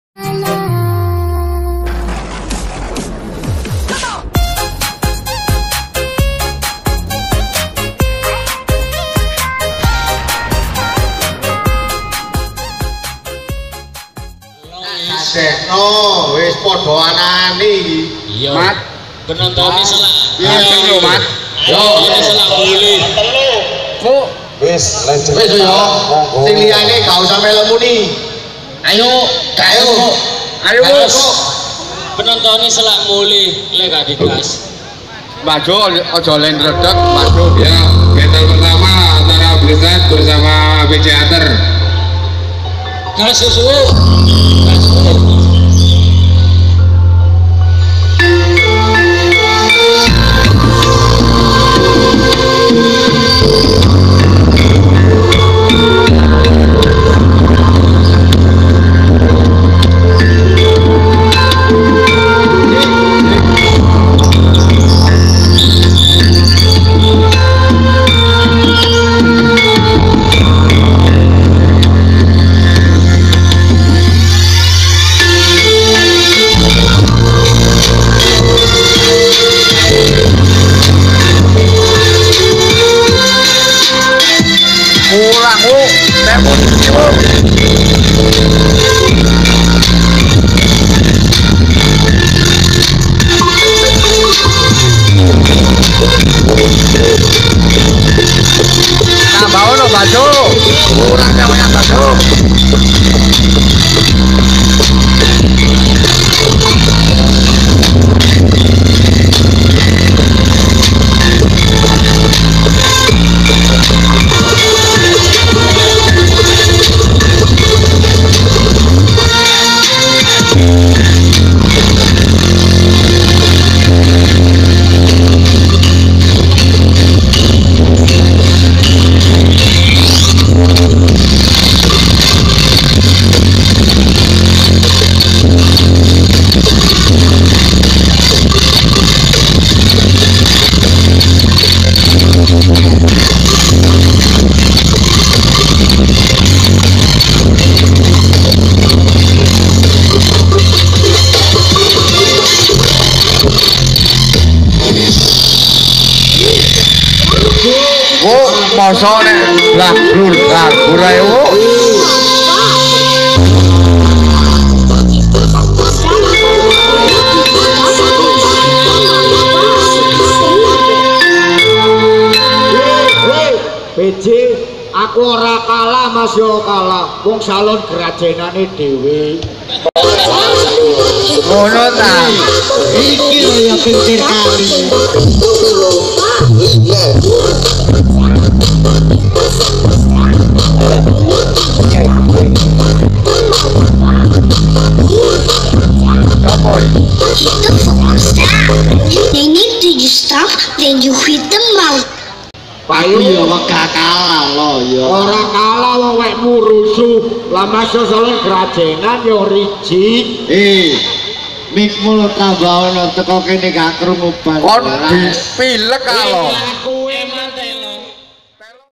BATTLE BJ HUNTER VS BLIZZARD //BATTLE REMBUN DAMPIT 2020//
Inilah awal battle di mulai antara Bj hunter vs blizzard audio.

"Battle di desa rembun dampit Malang 2020"